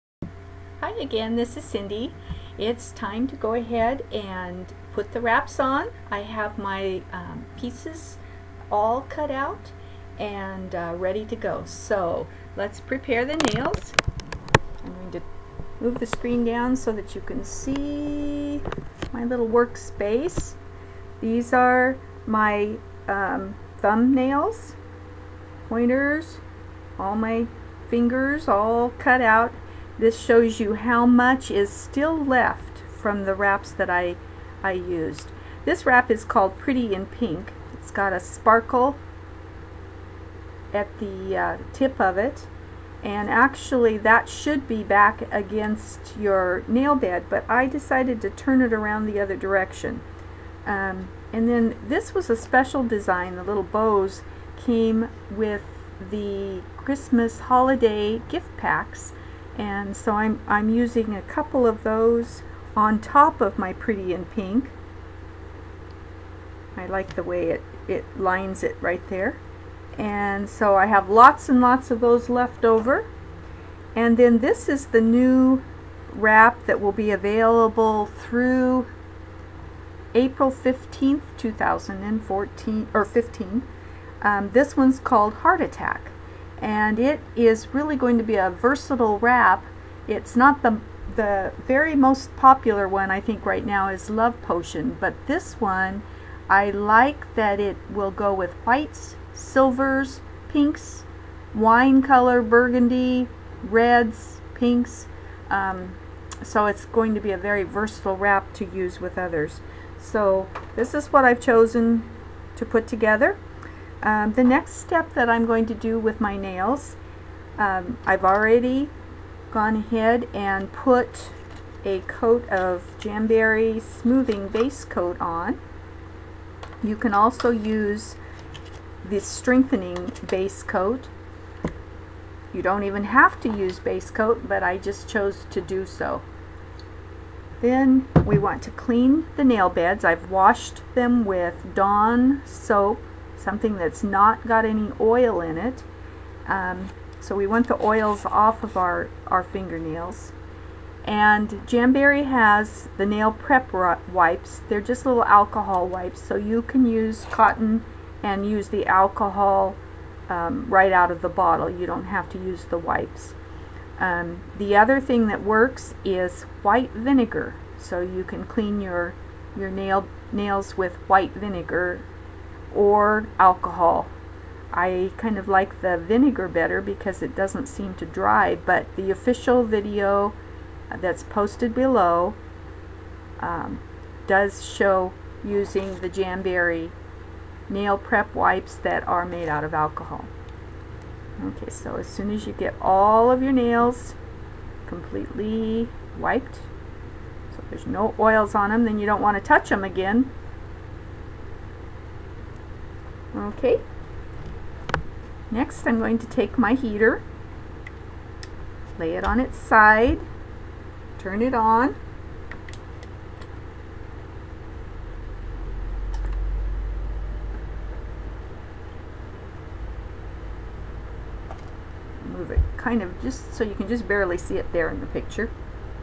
7 Ready to Apply wraps 4min
Here's a short video demonstrating a method of applying Jamberry Nail Wraps. Jamberry's official video is a must-watch first but here I am with a technique that also works well.